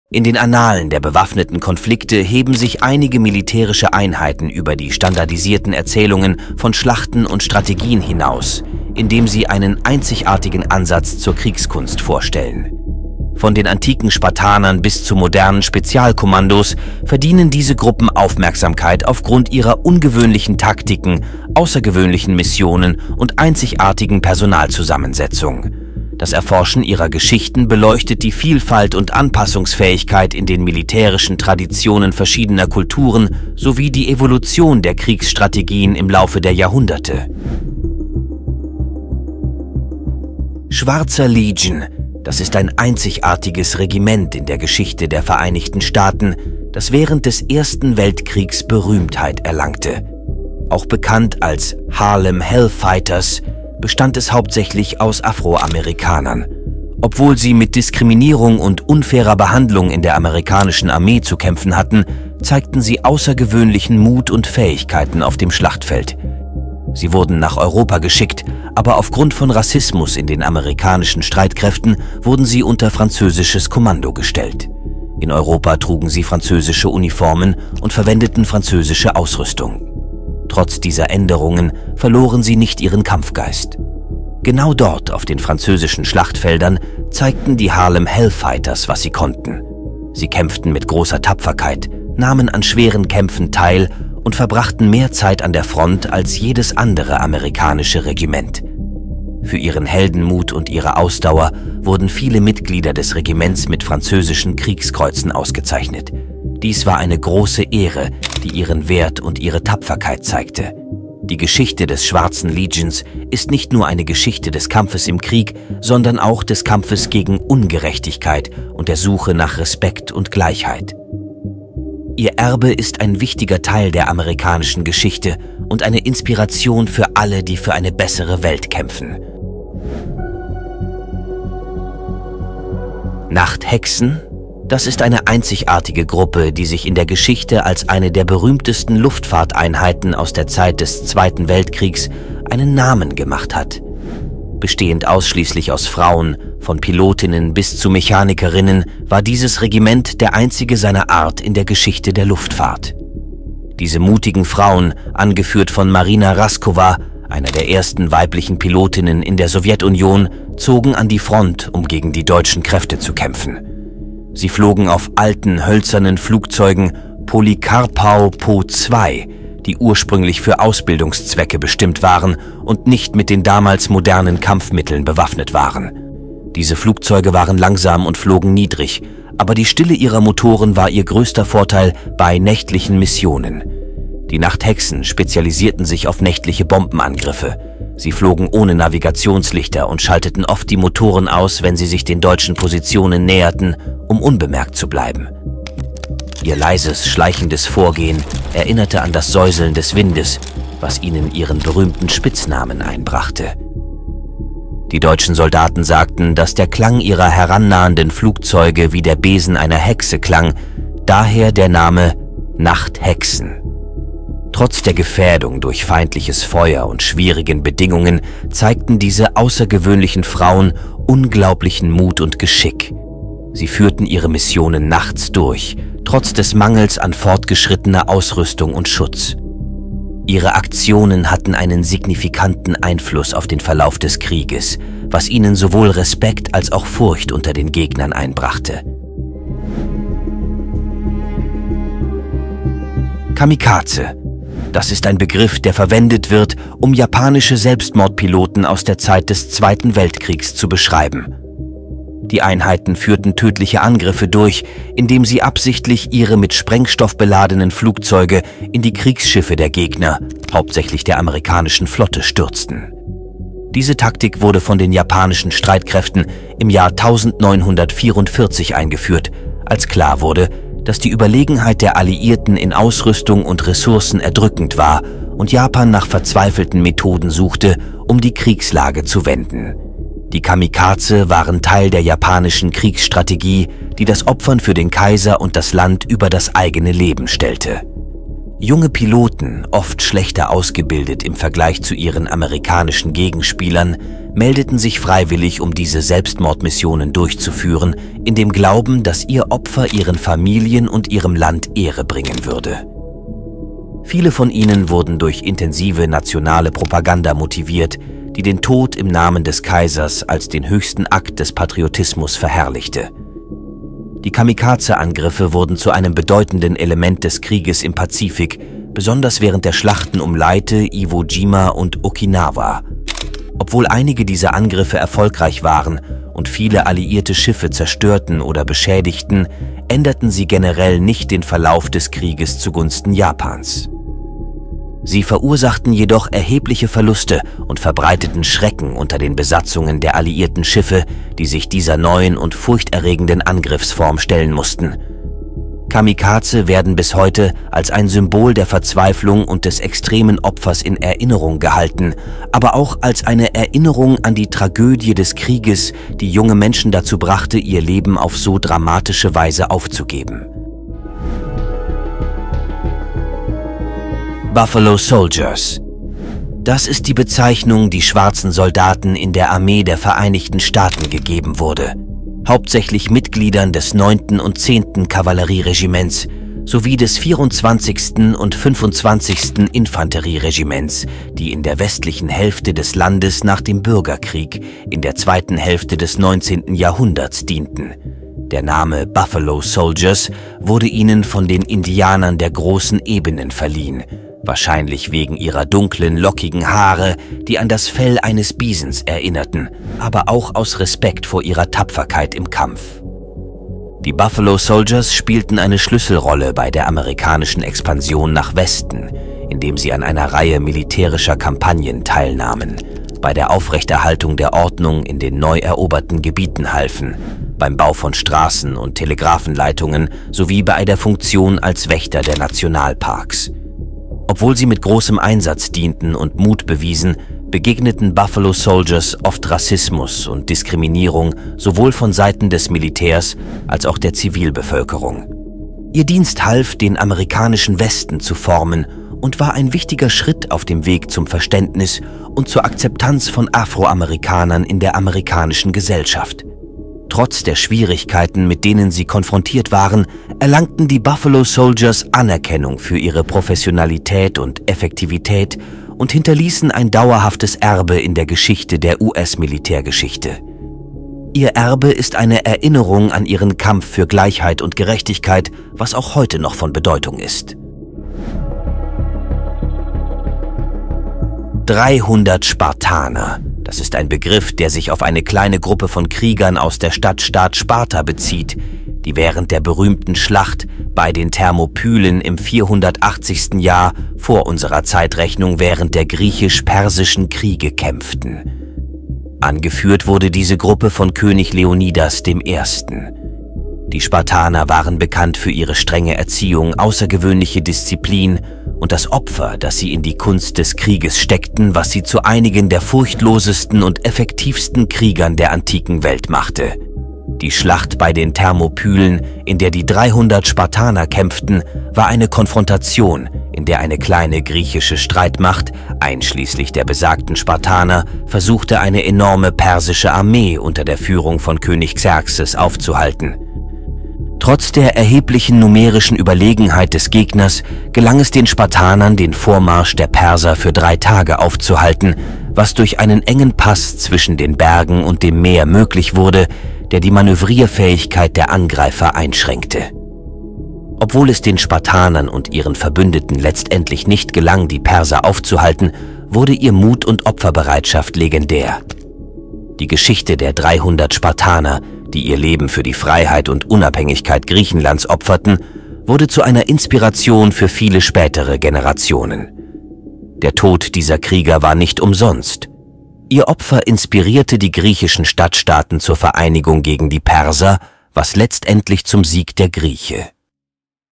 Die ungewöhnlichsten militärischen Einheiten in der Geschichte!
In den Annalen der bewaffneten Konflikte heben sich einige militärische Einheiten über die standardisierten Erzählungen von Schlachten und Strategien hinaus, indem sie einen einzigartigen Ansatz zur Kriegskunst vorstellen. Von den antiken Spartanern bis zu modernen Spezialkommandos verdienen diese Gruppen Aufmerksamkeit aufgrund ihrer ungewöhnlichen Taktiken, außergewöhnlichen Missionen und einzigartigen Personalzusammensetzung. Das Erforschen ihrer Geschichten beleuchtet die Vielfalt und Anpassungsfähigkeit in den militärischen Traditionen verschiedener Kulturen sowie die Evolution der Kriegsstrategien im Laufe der Jahrhunderte.

Klicke auf die 🔔, um Benachrichtigungen über die neuesten Videos zu erhalten!

Titel ► Die ungewöhnlichsten militärischen Einheiten in der Geschichte!